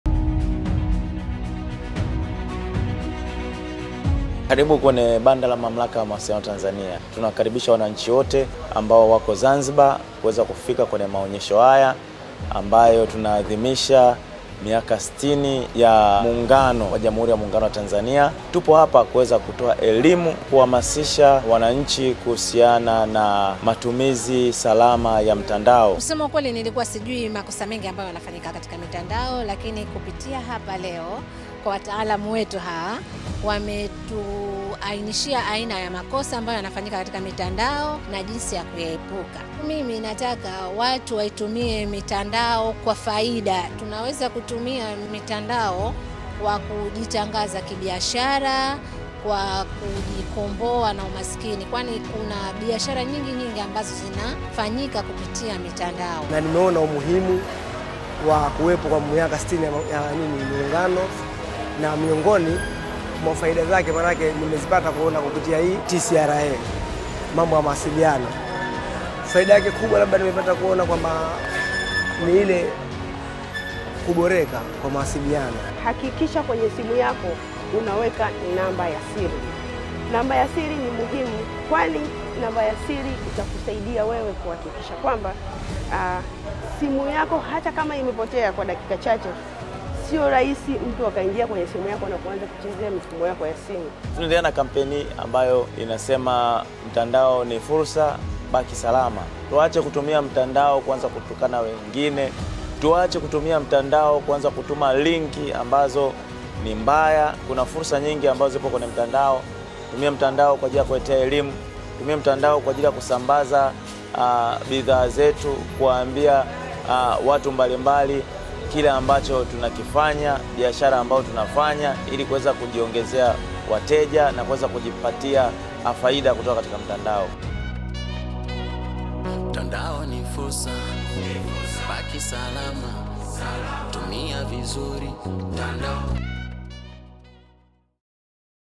Wito wa TCRA Umma kutoka Zanzibar "Mtandao ni Fursa Baki Salama"
Mamlaka ya Mawasiliano Tanzania (TCRA) inaendelea kushiriki katika maonesho ya kuadhimisha miaka 60 ya Muungano wa Tanganyika na Zanzibar  katika viwanja vya Nyamanzi, Zanzibar.